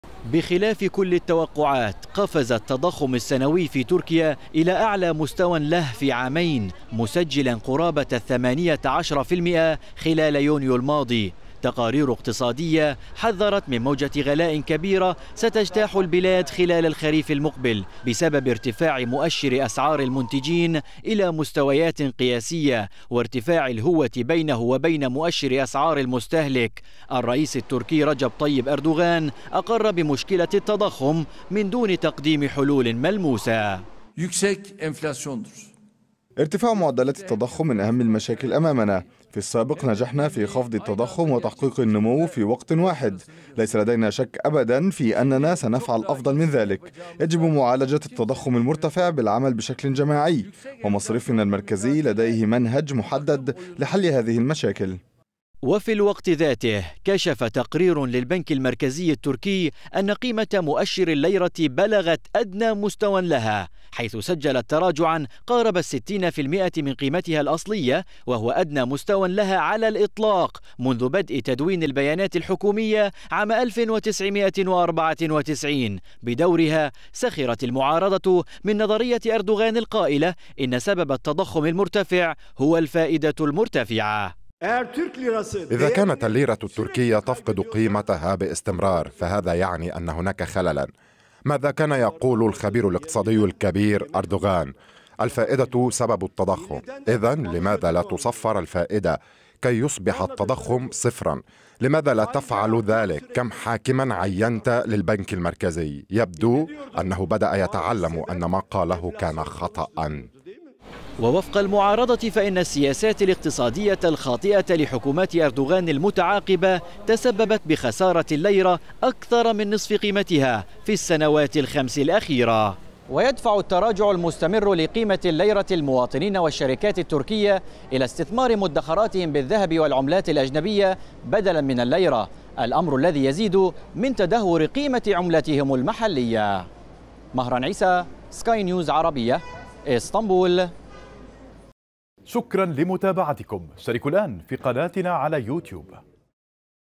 المعارضة التركية تحمل الحكومة مسؤولية الأزمة الاقتصادية
سجلت معدلات التضخم في تركيا مستوى قياسياً، حيث أظهر بيانات البنك المركزي تراجع القيمة الحقيقية للعملة إلى مستوى قياسي.
وتحمّل المعارضة حكومة العدالة والتنمية المسؤولية عن الأزمة الاقتصادية التي تعيشها البلاد.